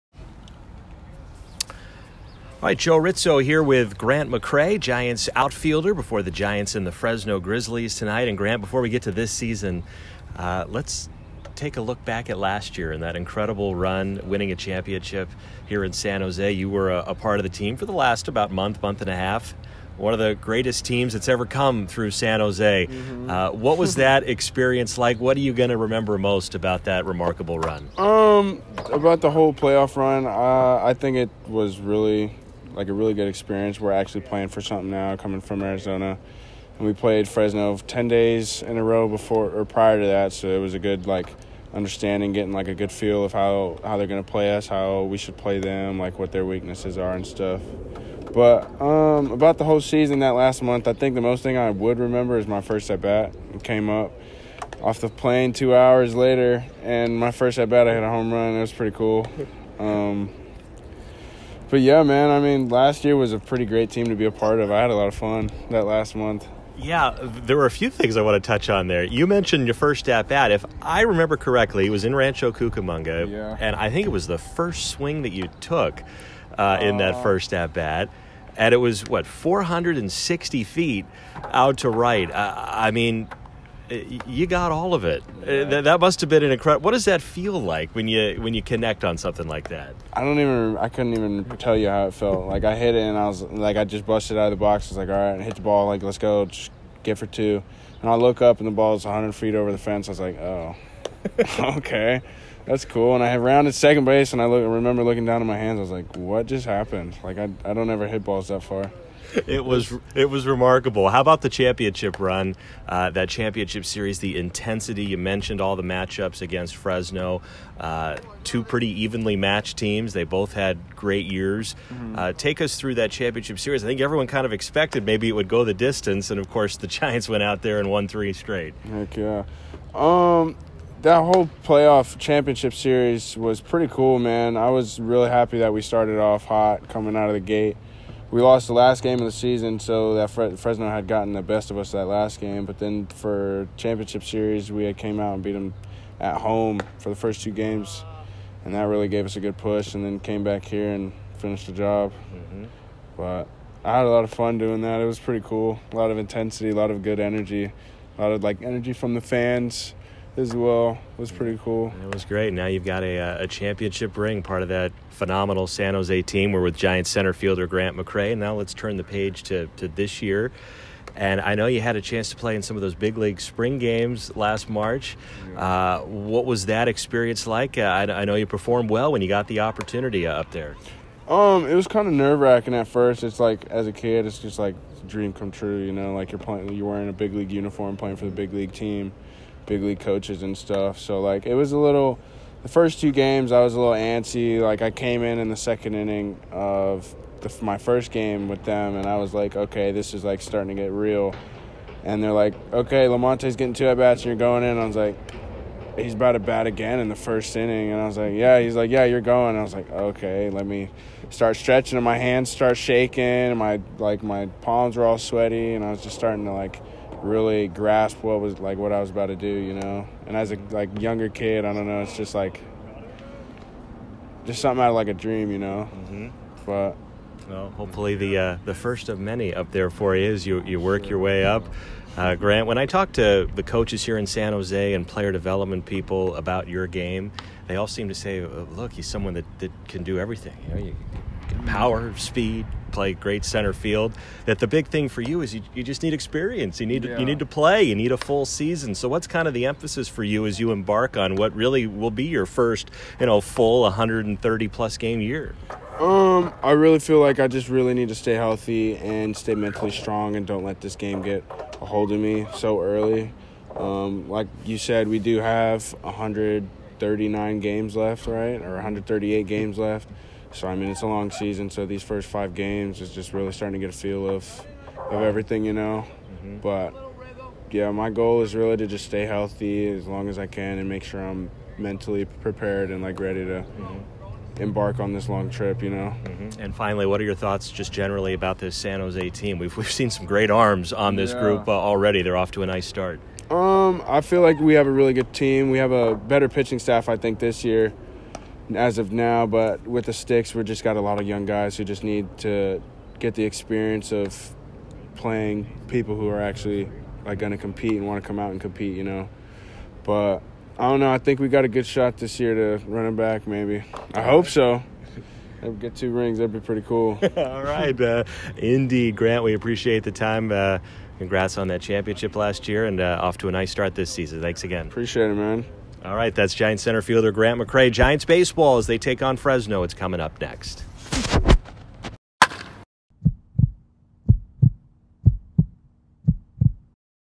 Joe Ritzo's Pregame Interview with Grant McCray - April 14, 2022
Outfielder Grant McCray discussed the impact of the 2021 Championship run on himself and how he hopes to build on last years success in San Jose again this year.